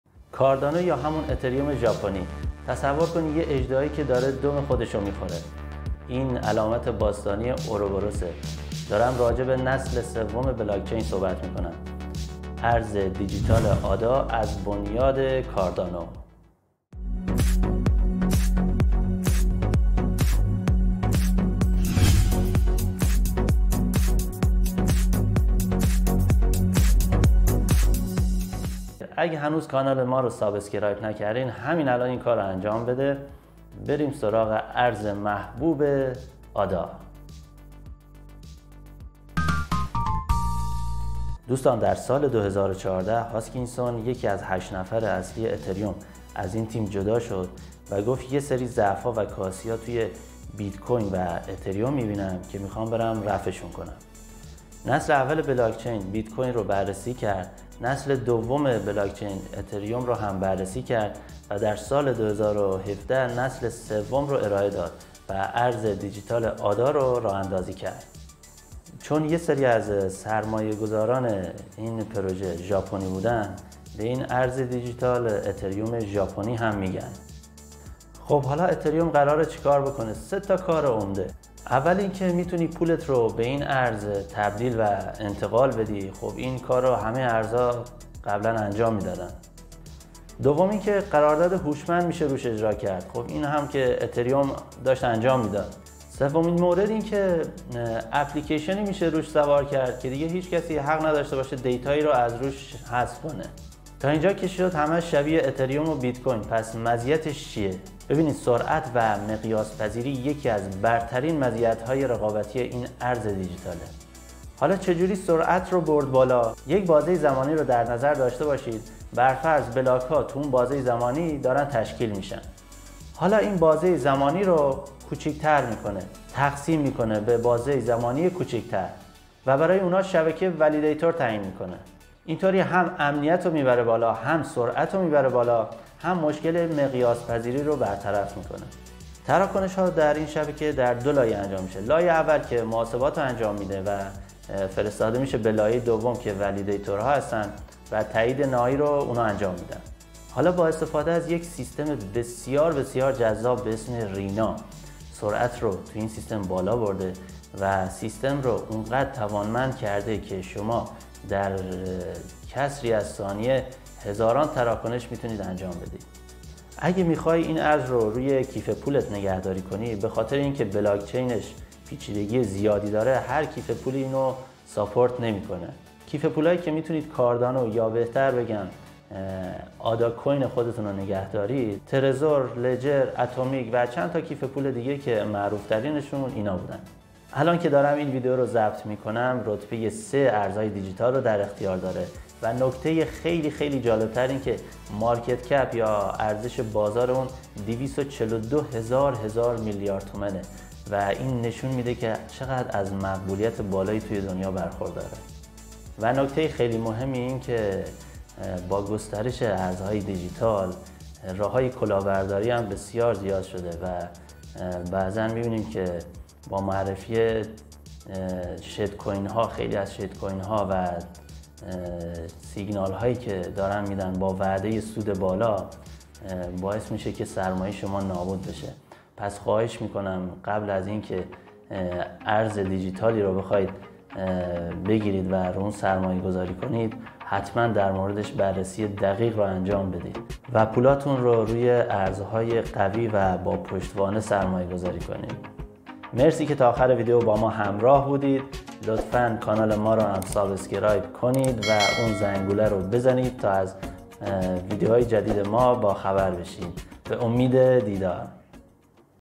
ارز دیجیتال ADA /بنیاد کاردانو
اگه میخوای با ارز دیجیتال آدا آشنا بشی و بدونی چه ویژگی هایی داره که هوادار زیادی پیدا کرده حتما این ویدیو رو تا آخر ببین